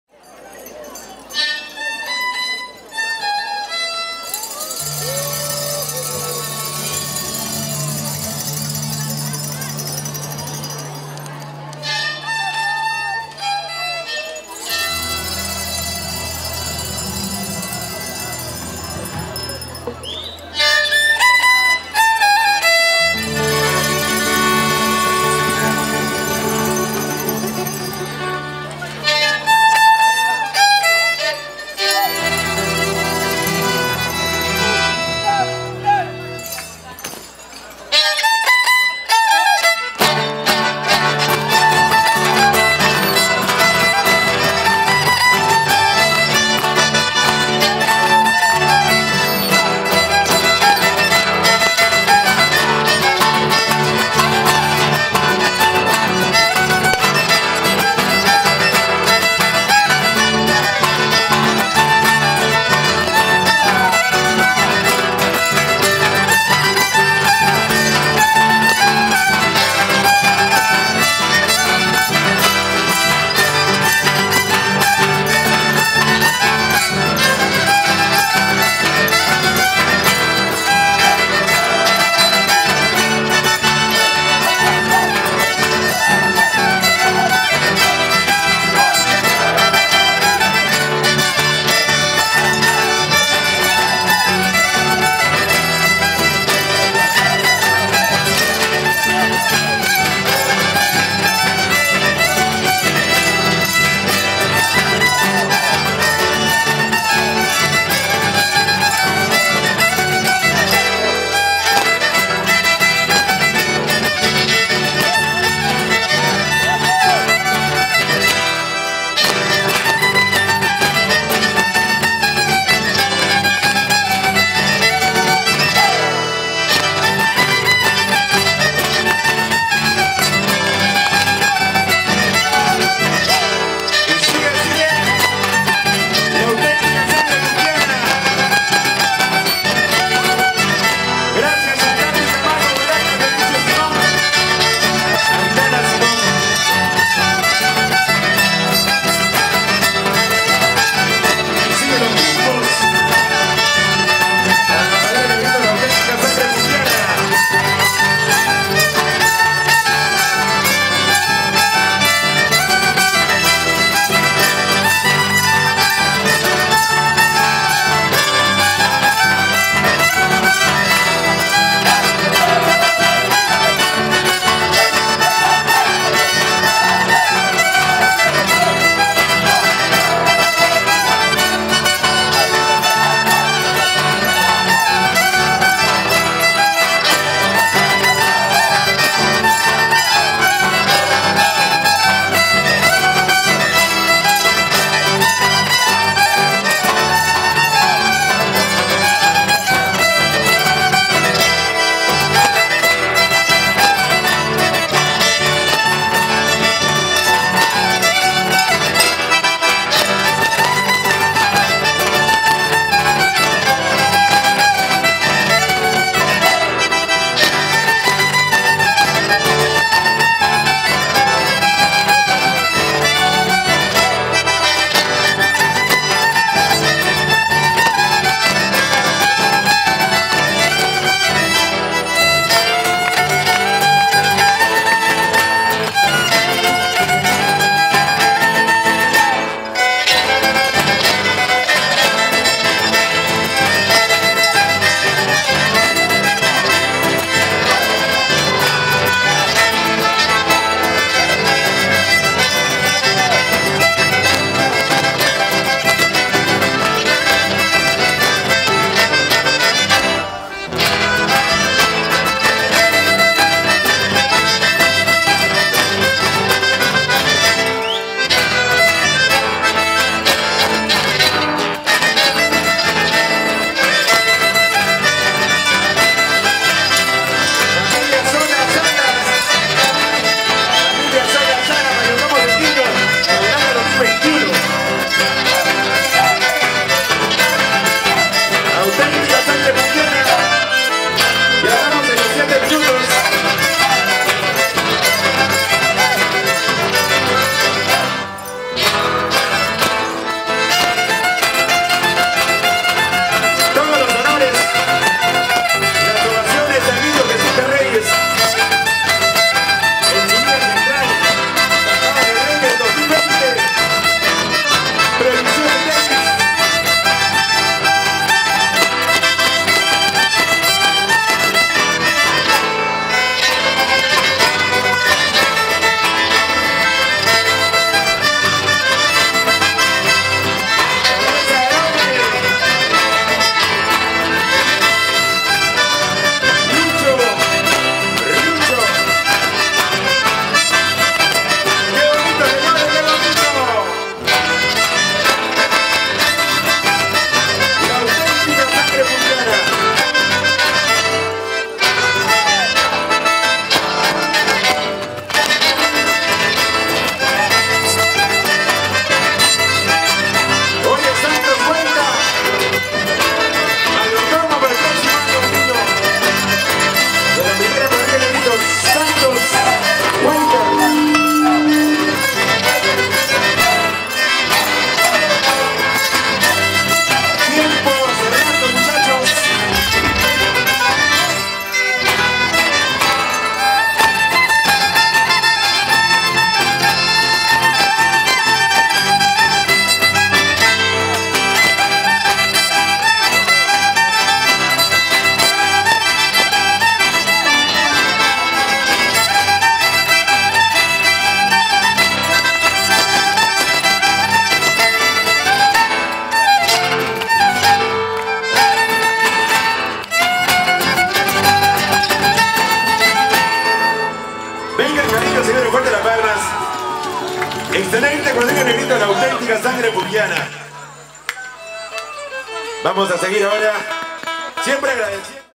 Bajada de Reyes 2020 ► Negritos de Puquio ► La Auténtica Sangre Puquiana ► Cuarta presentación
Bajada de Reyes 2020 en Nasca - Ica - Perú.
Bajada tradicional en honor al Niño Jesús de Reyes.
Cuadrilla de Negritos La Auténtica Sangre Puquiana de Puquio
CUARTA PRESENTACIÓN